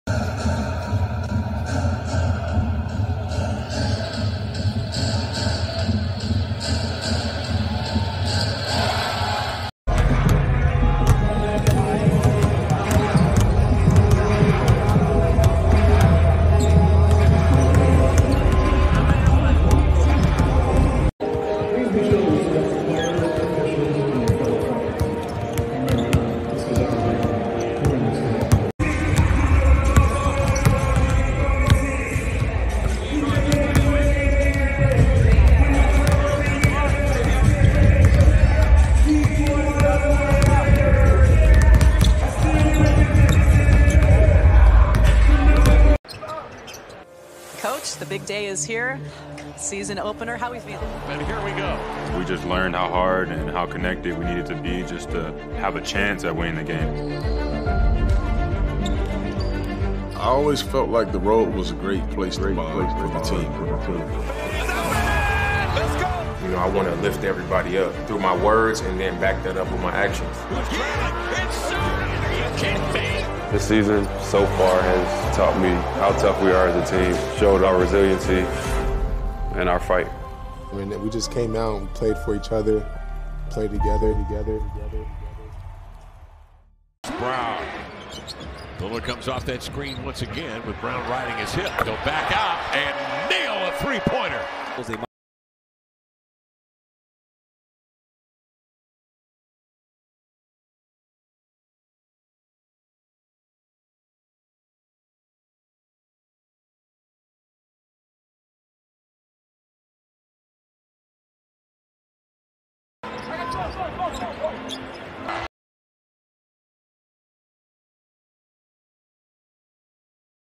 ANOTHER EXCITING WILD ENDGAME PORTLAND TRAILBALZERS HIGHLIGHTS AGAINST DENVER NUGGETS WIN 121-120
PORTLAND TRAILBALZERS HIGHLIGHTS